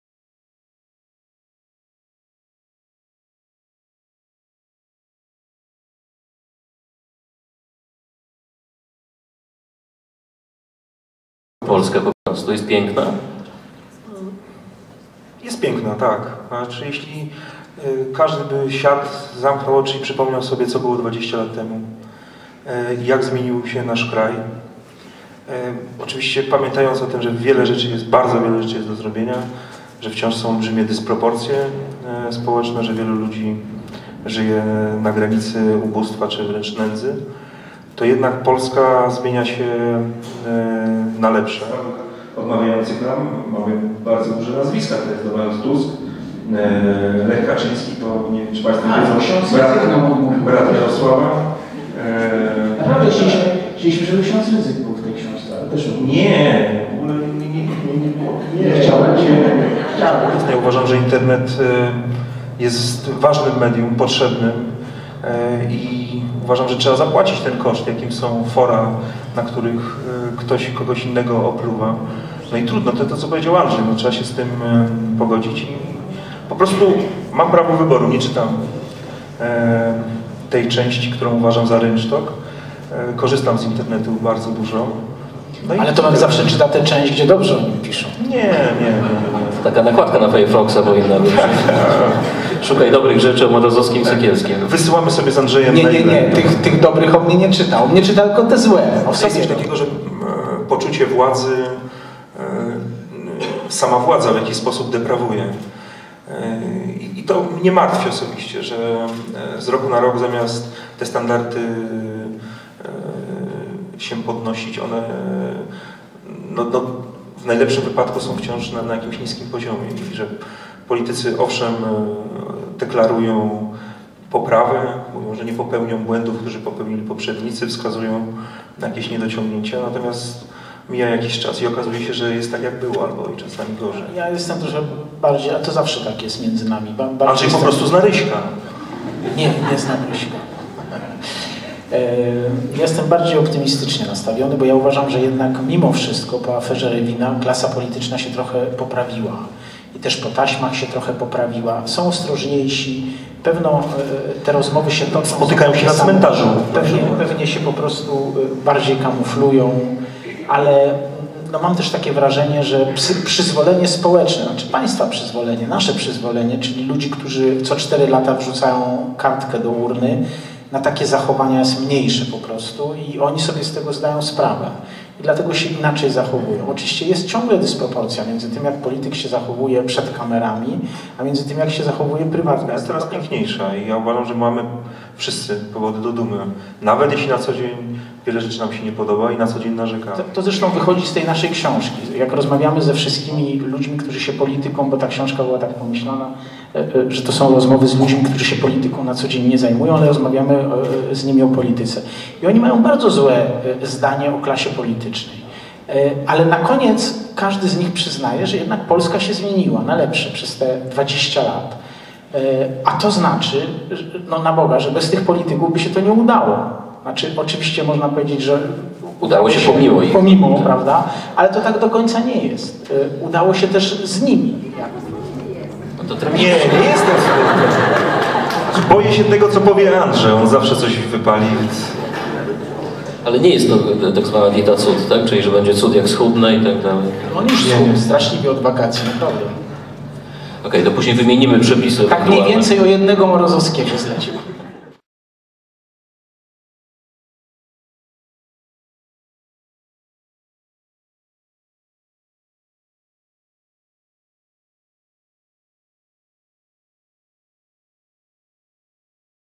Sekielski i Morozowski w drukarni Opolgraf
Spotkanie z cyklu "Fabryka Inspiracji" z Tomaszem Sekielskim i Andrzejem Morozowskim w drukarni Opolgraf